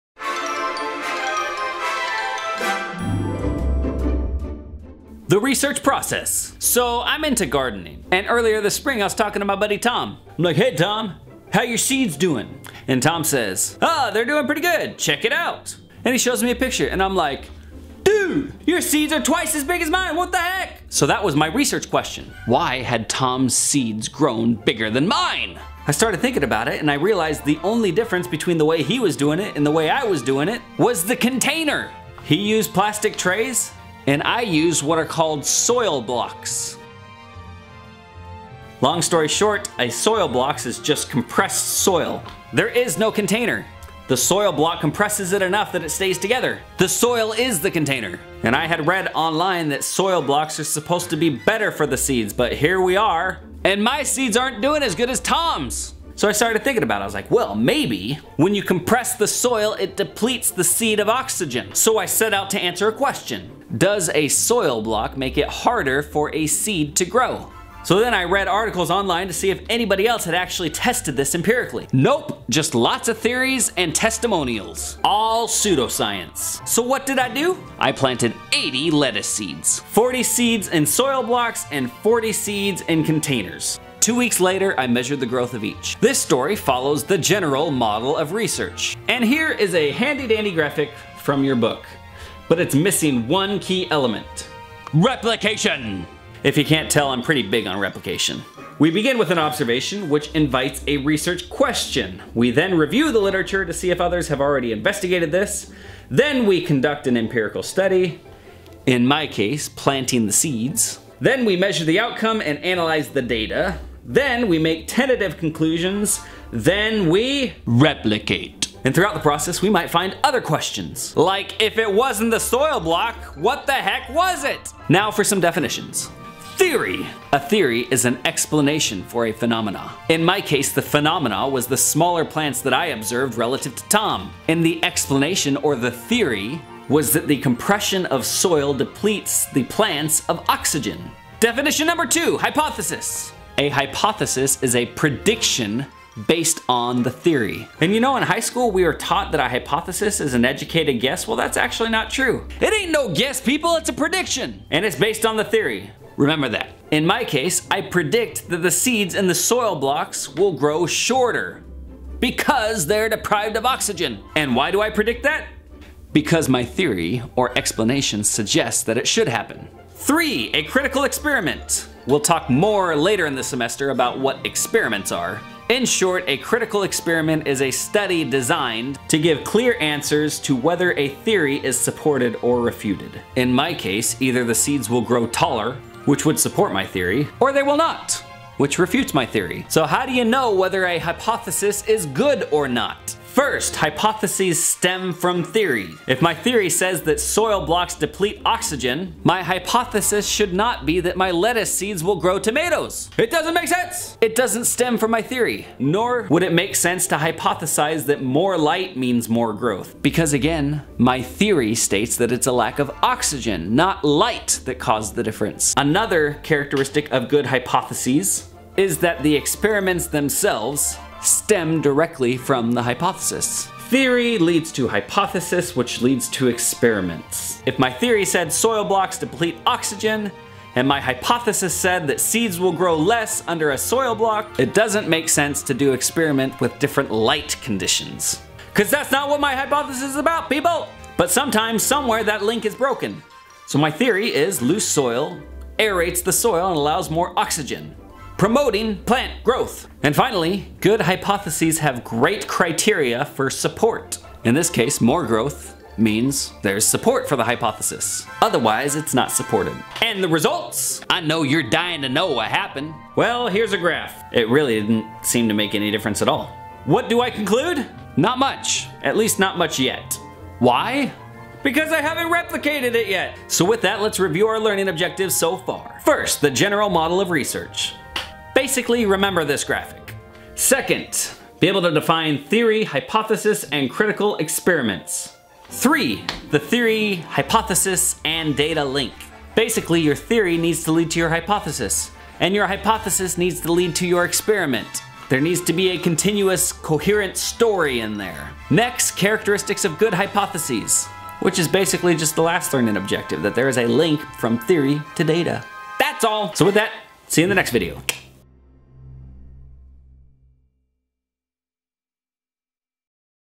The research process
objectives
1. The general model of research
2. Define theory, hypothesis, and cirtical experiments
3. Theory/hypothesis/data link
4. Characteristics of good hypotheses